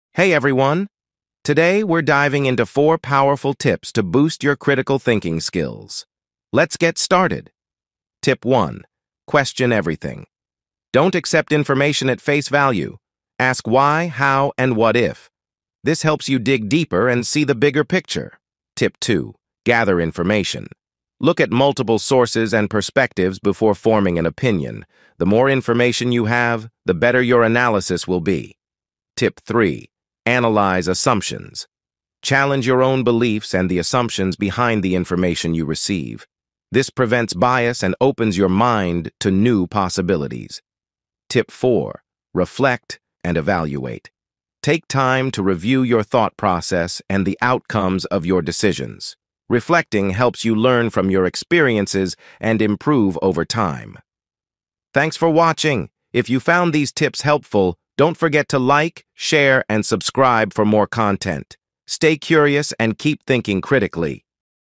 4 Tips to Improve Critical Thinking | Boost Your Decision-Making Skills
Unlock the secrets to sharper thinking and better decision-making! In this video, we reveal four essential tips to improve your critical thinking skills. Whether you're a student, professional, or lifelong learner, these strategies will help you analyze information more effectively and solve problems with confidence. Watch now to start thinking critically!

Timestamps:
0:00 Introduction
0:20 Tip 1: Question Everything
0:50 Tip 2: Gather Information
1:20 Tip 3: Analyze Assumptions
1:50 Tip 4: Reflect and Evaluate